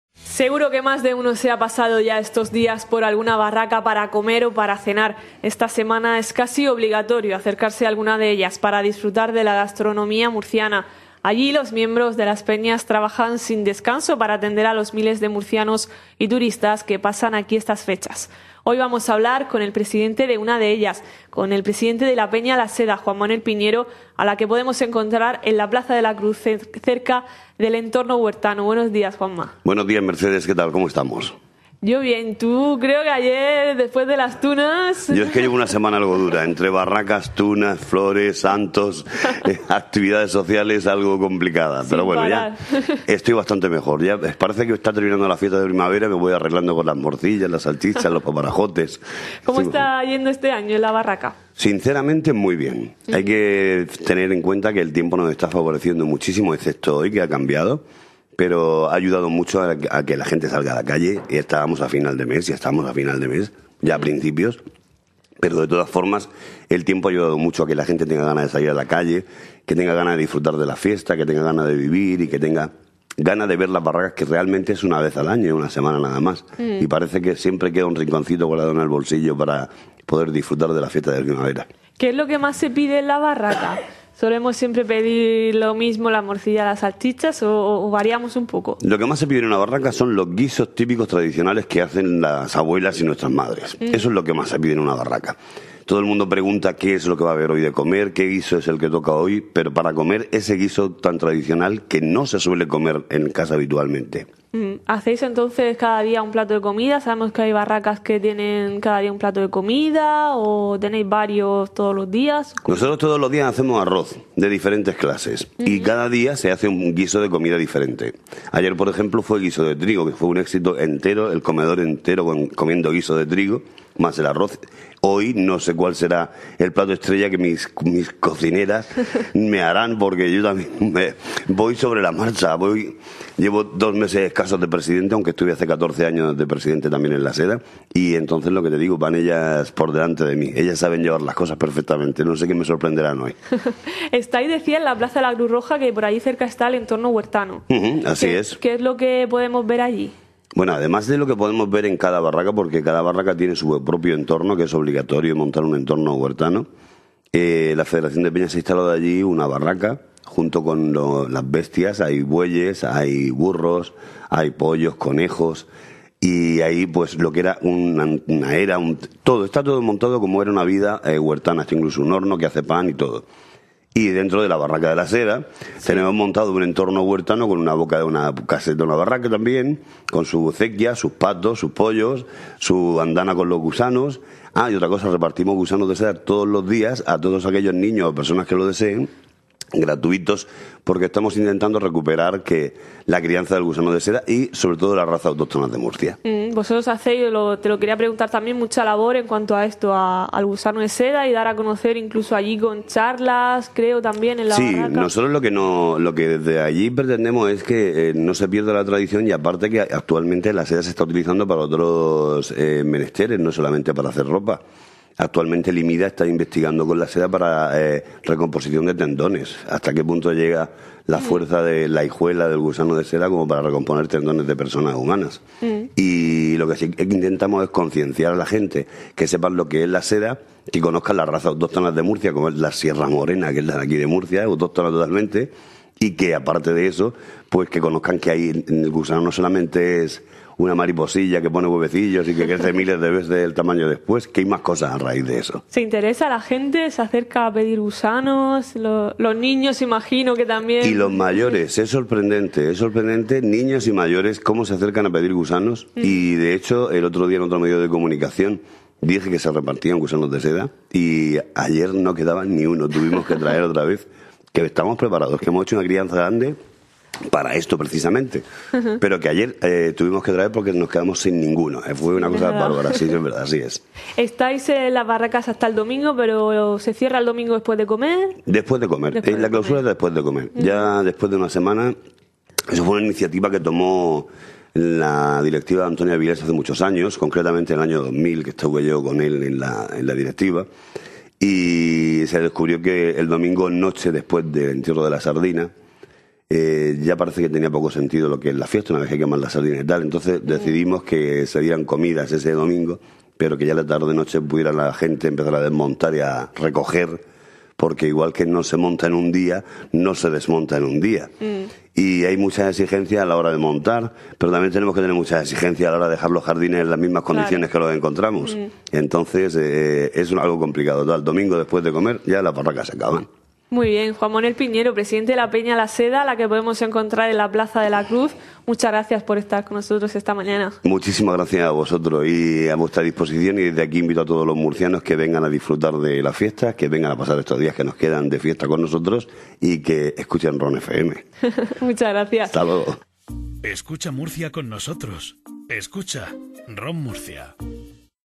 Entrevista al presidente de la Peña Huertana La Seda, en ROM Murcia
Entrevista realizada en Radio Online Murcia a D. Juan Manuel Piñero Marín, presidente de la Peña Huertana La Seda.
1 de abril de 2016